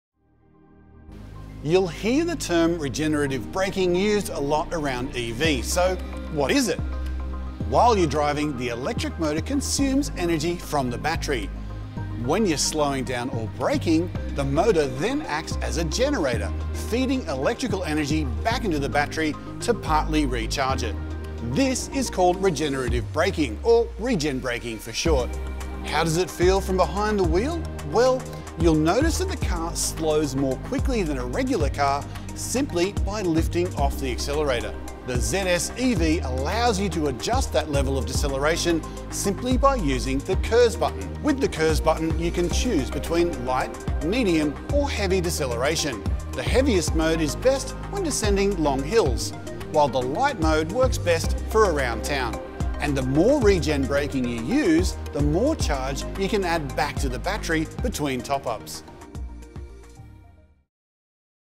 How does the MG ZS EV regenerative braking work?
Electric vehicles are shaping the future of motoring! Europcar aims to be the car rental leader in this emerging market and an advocate for the use for electric vehicles.